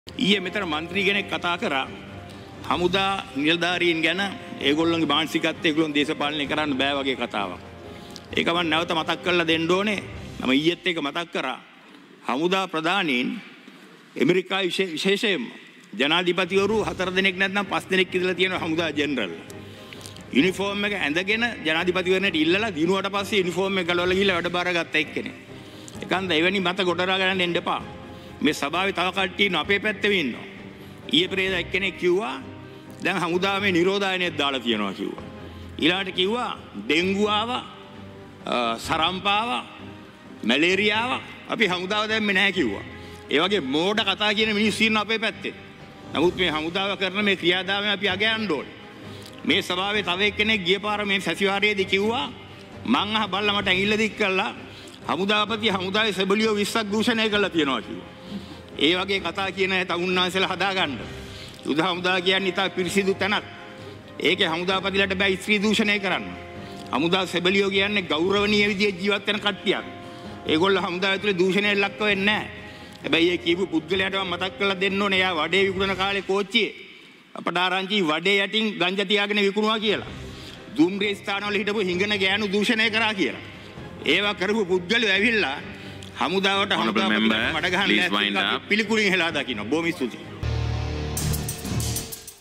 සරත් ෆොන්සේකා සෙබලියෝ 20ක් දුෂණයකලා කියන කථාවට පිළිතුරු දෙයි | Breaking.lk
සරත් ෆොන්සේකා සෙබලියෝ 20ක් දුෂණයකලා කියන කථාවට පිළිතුරු දෙයි